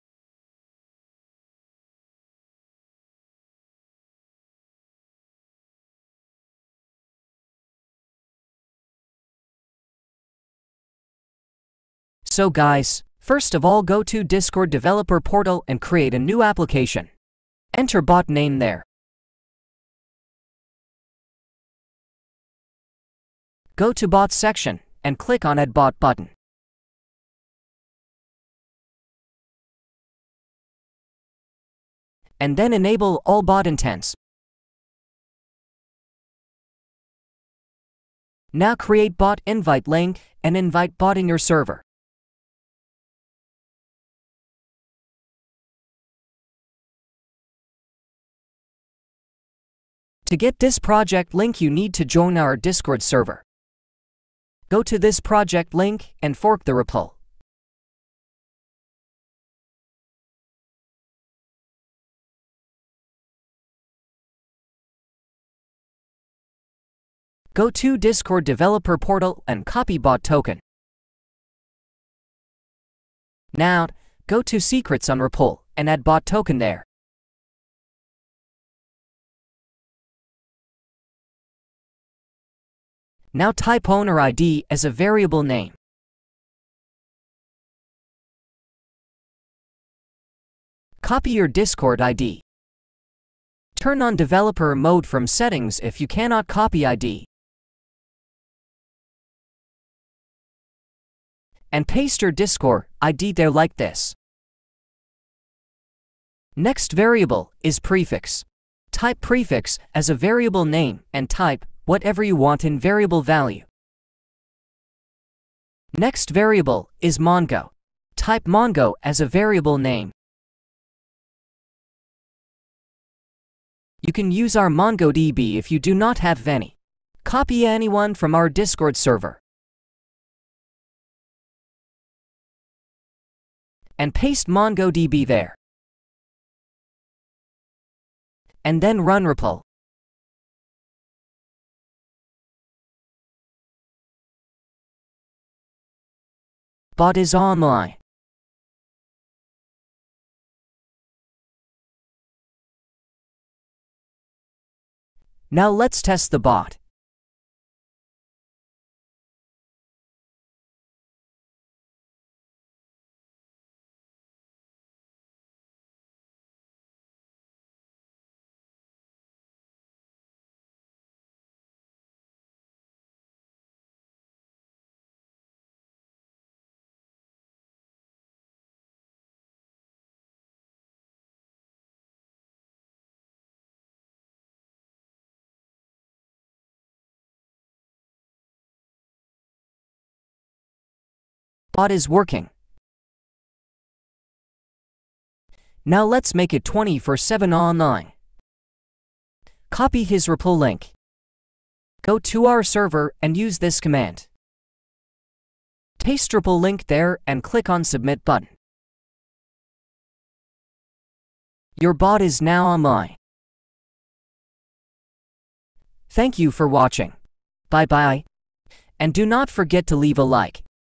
🔥 How To Make Discord Bot + 100+ Commands + No Coding Required 🥵
In this video, we'll be showing you how to create a Discord bot with over 100 commands, without any coding required. We'll be using an all-in-one Discord bot platform that allows you to easily set up and customize your own bot, without any prior programming experience.

By the end of this video, you'll have a fully functional Discord bot that can perform a wide range of tasks and functions, making it a valuable asset for your Discord community. So whether you're a server admin, a content creator, or just someone looking to add some extra fun to your Discord chats, this video is for you.

💘 Emotes Servers - feel free to invite you bot there